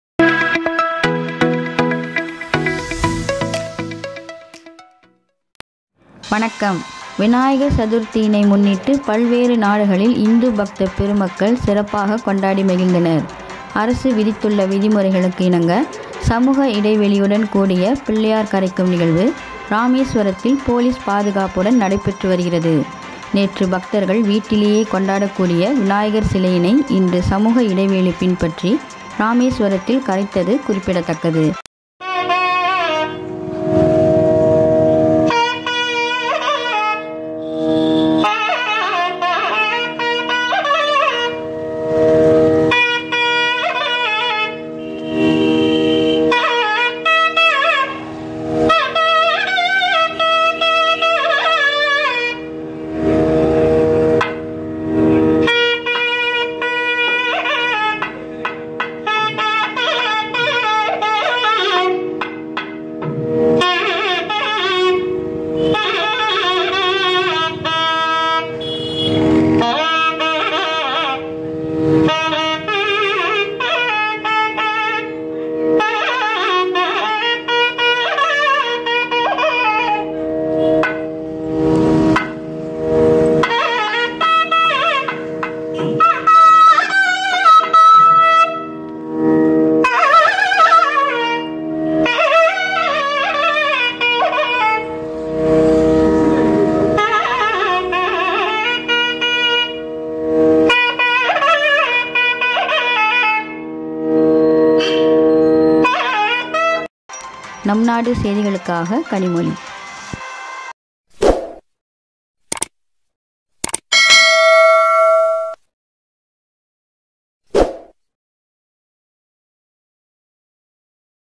இராமேஸ்வரம் கடலில் போலீஸ் பாதுகாப்புடன் விநாயகர் சதுர்த்தி | breaking news today
விநாயகர் சதுர்த்தியினை முன்னிட்டு பல்வேறு நாடுகளில் இந்து பக்த பெருமக்கள் சிறப்பாக கொண்டாடி மகிழ்ந்தனர். அரசு விதித்துள்ள விதிமுறை இனங்க சமூக இடைவெளியுடன் கூடிய பிள்ளையார் கரைக்கும் நிகழ்வு இராமேஸ்வரத்தில் போலீஸ் பாதுகாப்புடன் நடைபெற்று வருகிறது.நேற்று பக்தர்கள் வீட்டிலேயே கொண்டாடிய விநாயகர் சிலையினை இன்று சமூக இடைவெளி பின்பற்றி ராமேஸ்வரத்தில் கரைத்தது குறிப்பிடதக்கது.

👇VIDEO LINK